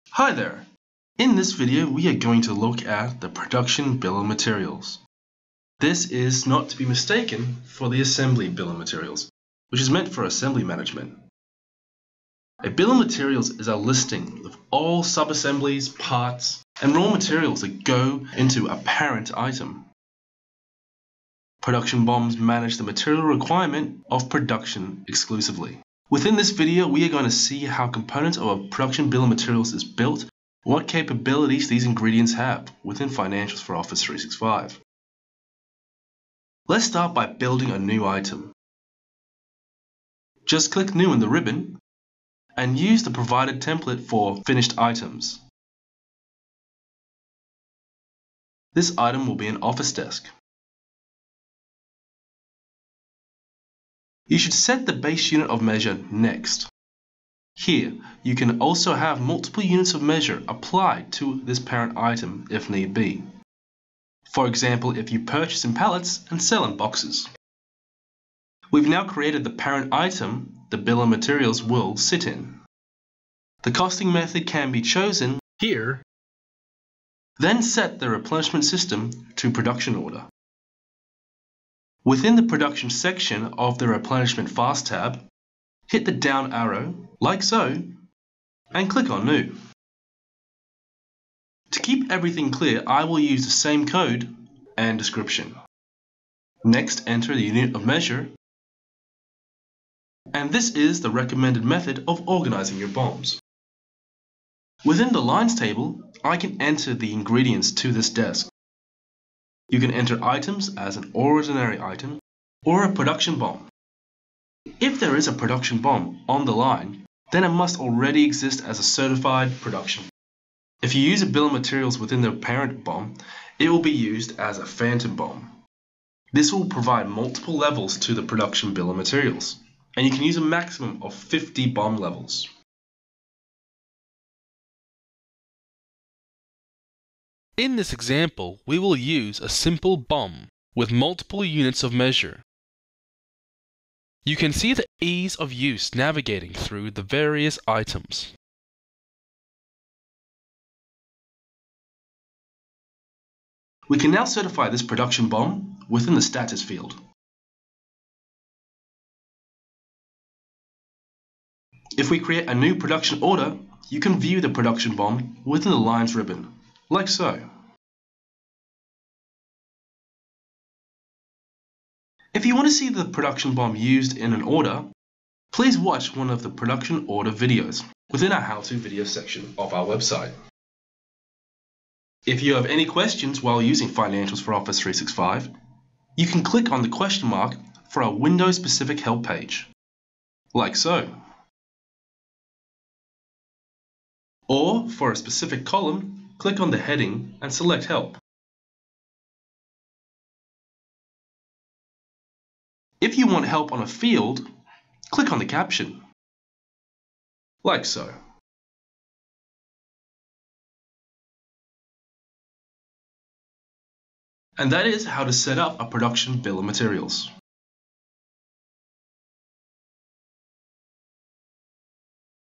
How to Set up Production Bill of Materials (BoM)
Welcome to How-to-Setup Production Bill of Materials. Here we will see  a Bill of Materials see how the components of a Production BOM is built, what capabilities these ingredients have.

A production BOM holds master data that describes the components and subassemblies used in the production of a parent item. Once a production order is created for that parent item, its production BOM will govern the calculation of material requirements as represented in the Prod. Order Components window.

Prerequisites
- Item cards are created for parent items and their components.
- Gather all data about item structures: Top-level items, subassemblies, components, and their interrelation.